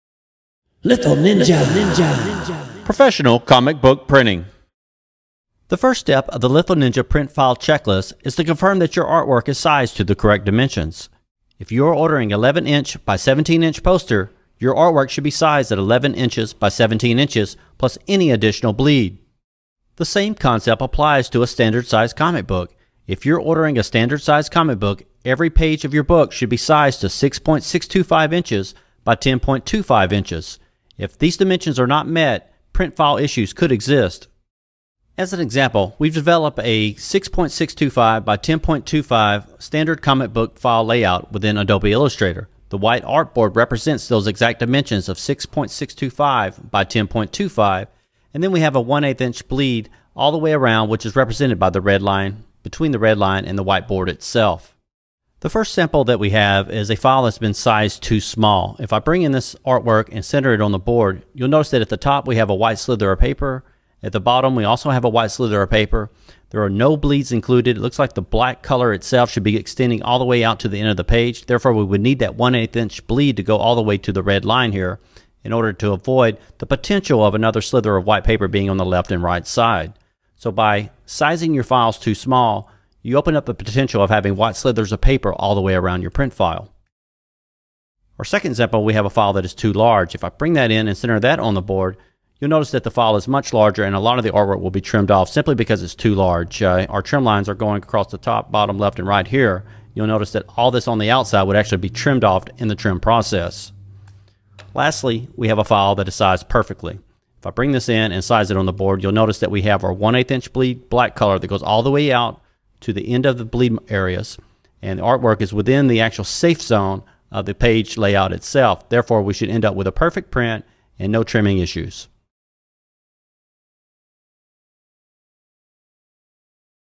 Litho Ninja Tutorial : Comic Book Dimensions
Details and explanation regarding Litho Ninja Comic Book Printer's print dimensions and comic book dimensions in general. Litho Ninja Comic Book Printer's dimensions located